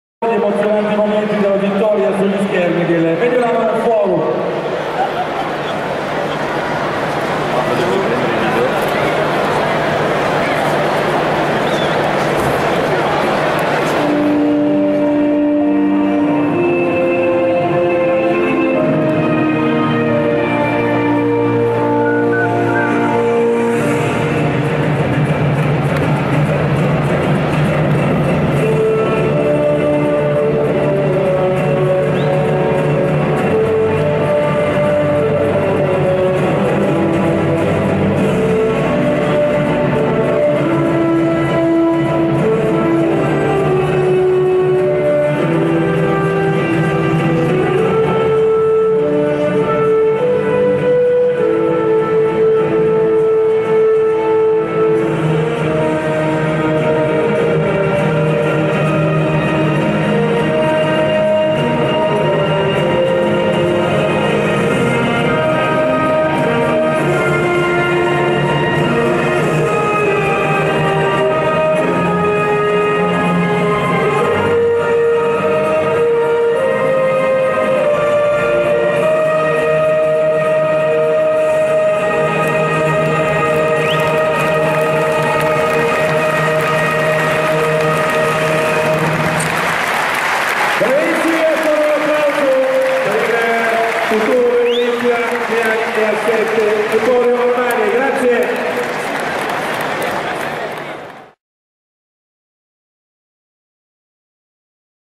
I Campioni d'Italia premiati al Forum
Momento emozionante per i ragazzi classe '97, premiati all'intervallo della partita fra Olimpia EA7 e Cimberio Varese. Per loro l'applauso degli 11.000 del Forum.